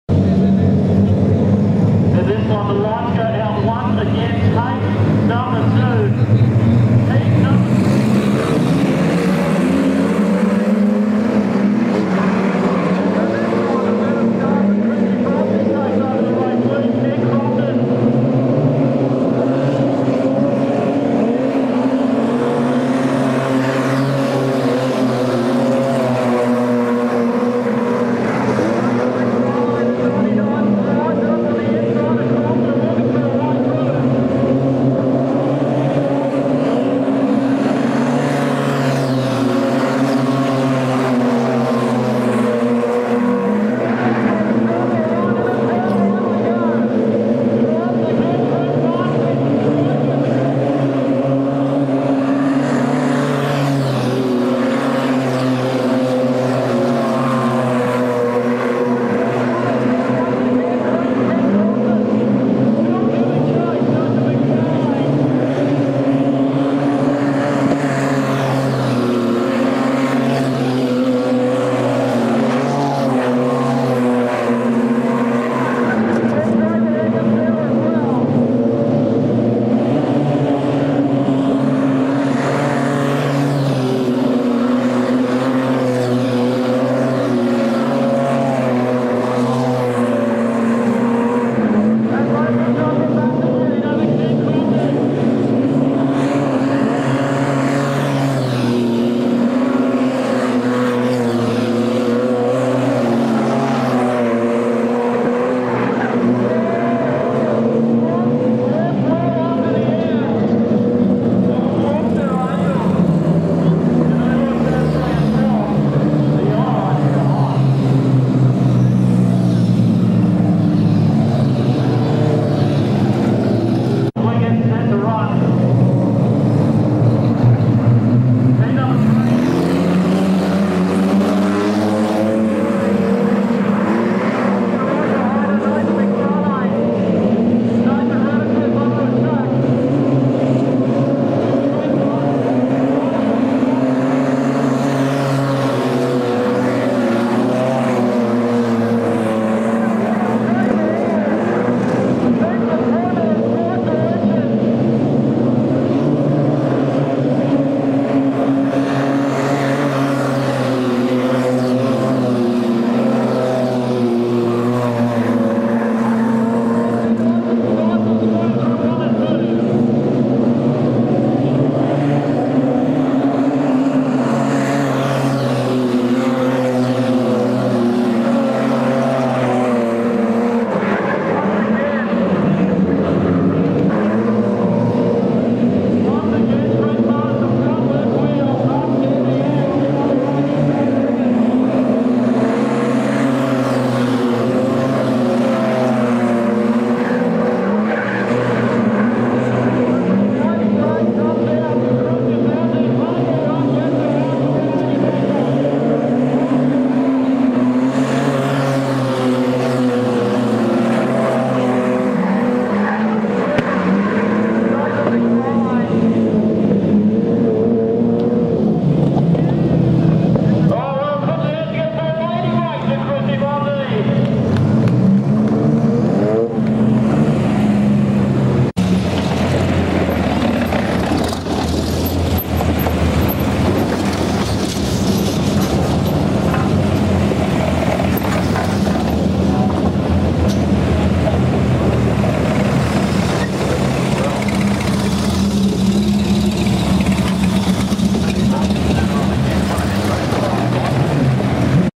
WINGLESS SPRINTS QLD TITLE.HEAT & GREAT WIN BY KRISTY BONSEY (WOMEN IN SPEEDWAY)27th APRIL 2013
Wow what a heat with Kristy Bonsey hold all the others out to take a great Win.This was a top drive.Congratulations Kristy.